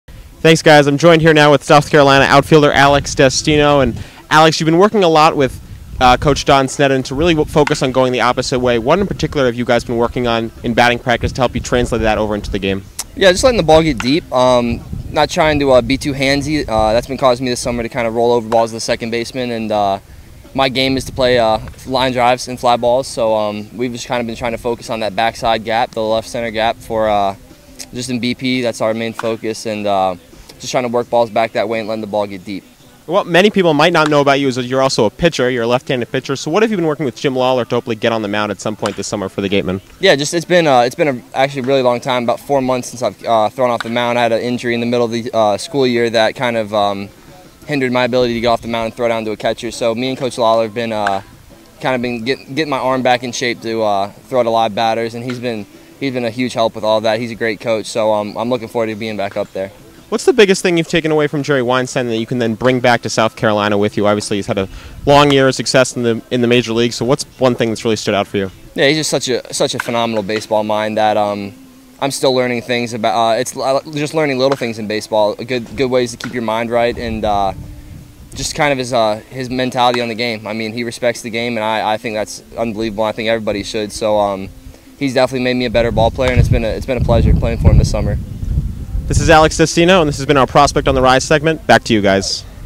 7-18-2016 Prospect on the Rise: Alex Destino
Matt Feld talks with Gatemen outfielder Alex Destino about working on hitting to all fields, his pitching ability and more!